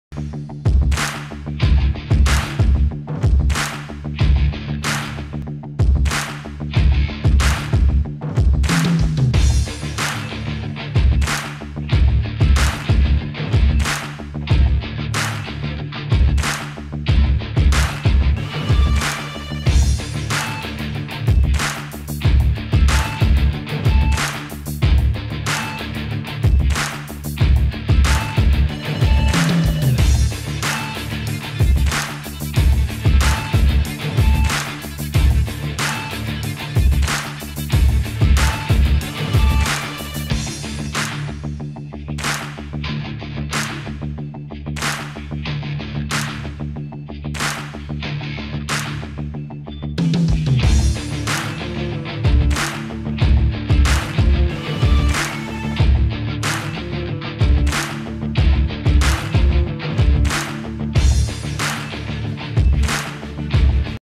How To Check Folder size in Windows 10 PC Laptop - Know any Windows folder Size Easily
Sometimes you may need to know  Windows folder Size and the size of any files and  folder in your windows PC or Laptop. In this video you will learn how to Check Folder size in Windows 10 Computer .
Simply , hover over the folder , you want to know the file size . alternatively , you can right click on the expected folder and hit the properties option .
You will see a new pop up window with the folder size details.